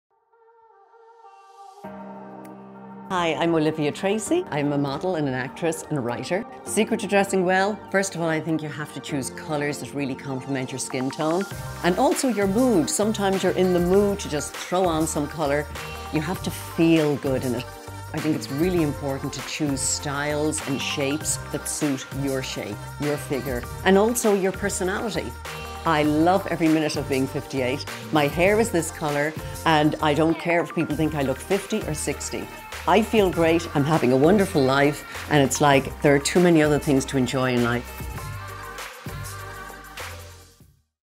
Kildare Village - Your Style, Your Way - Olivia Tracey
Kildare Village invites you to discover your style, your way this Autumn.
Introducing Indy, Angela, Annmarie and Olivia - these four amazing, smart and sophisticated women are our style crushes this season. They speak to us about how their style has evolved and grown with them throughout their lives!